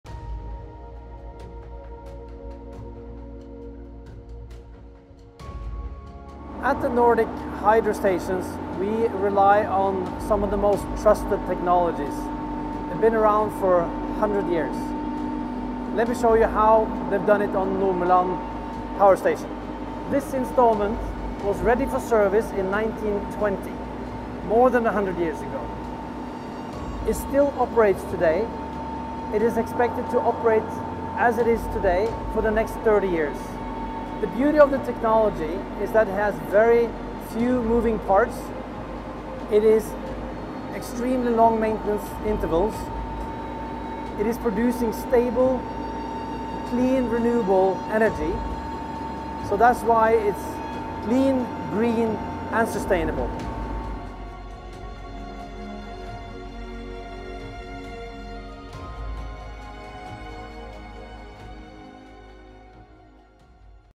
Green Technology That's Built To Last | Bulk Infrastructure
Peder Nærbø, Bulk's Founder and Chairman, showcases Nomeland hydro station, one of our sites which has been fully operational for over 100 years. With trustworthy, long-lasting technology, it's no wonder were leading the charge in fully renewable digital infrastructure.